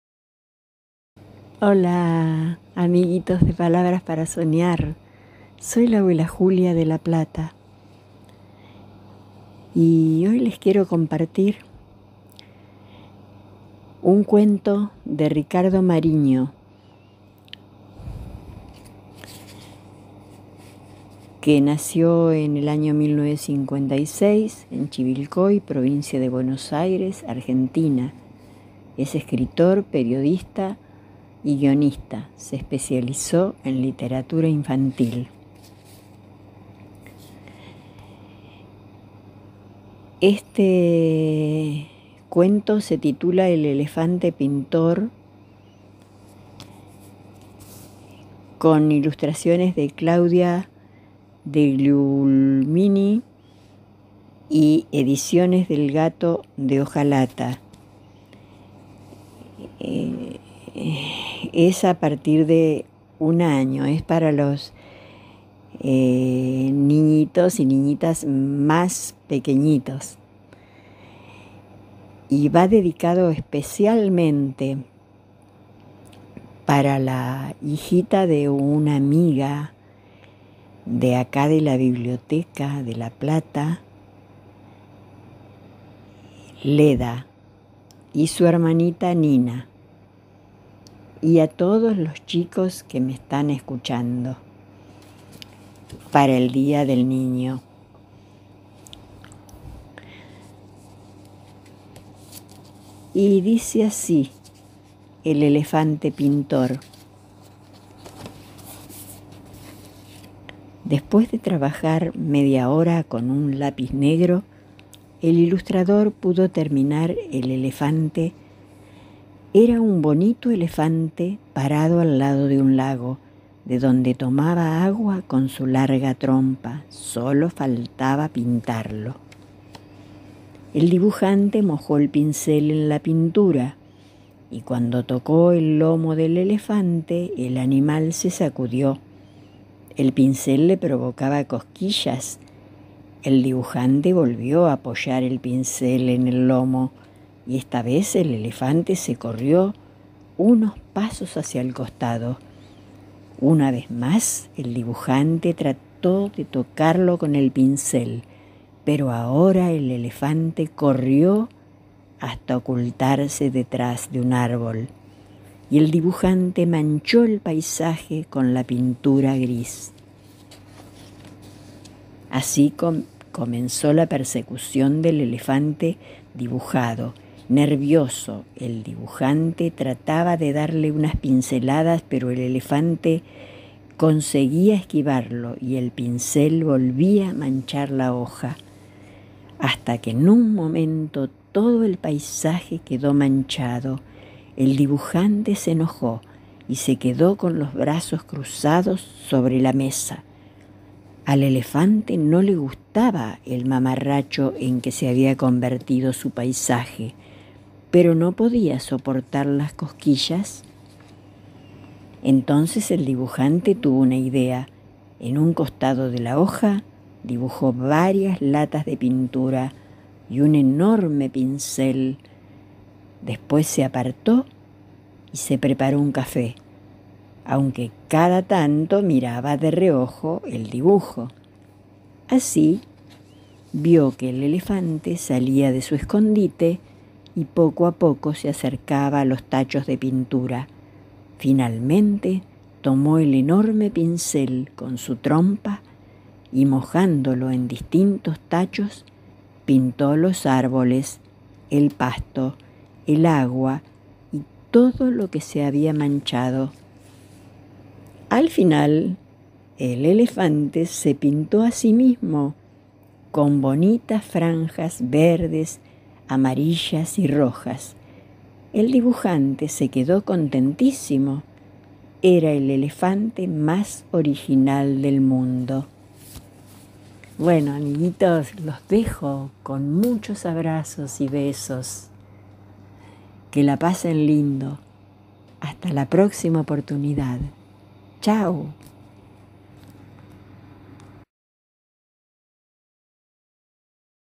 La abuela Julia nos lee "El elefante pintor", de Ricardo Mariño.
Ricardo Mariño (4 de agosto de 1956 - Chivilcoy, Provincia de Buenos Aires) es un escritor, periodista y guionista argentino, principalmente reconocido por sus cuentos para niños.